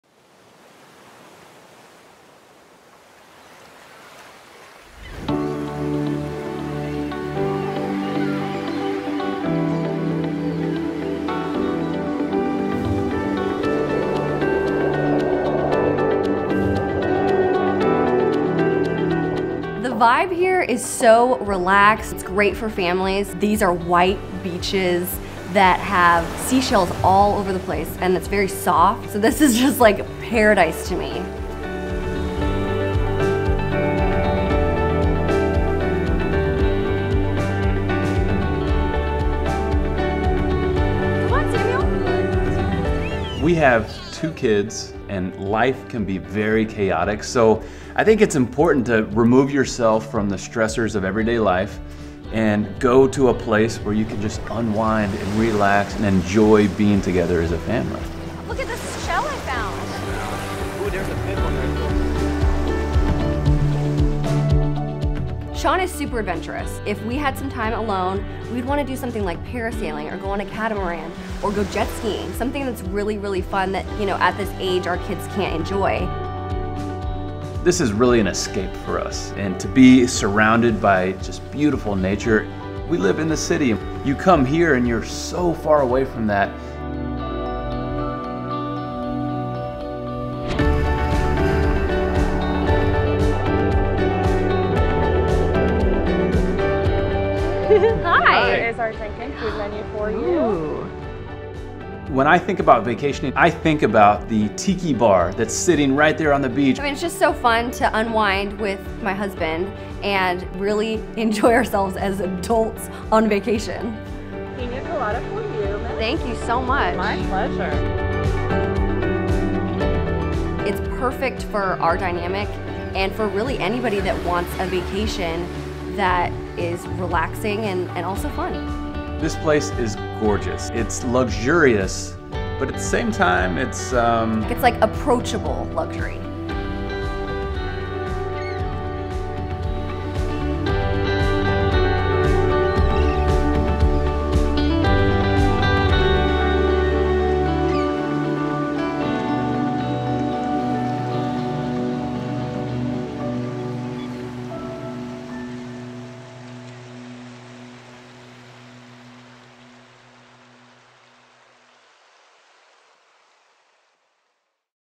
5 Days in Paradise: Adventurous Escape
Catherine and Sean Lowe escape the stresses of daily life, and find a place to relax and unwind on Florida’s Paradise Coast! The turquoise water and white sand beaches are a stunning sight; however, the vast array of water sports coupled with the scenic view really make Marco Island a place to experience! Find out more at discoveryourparadise.com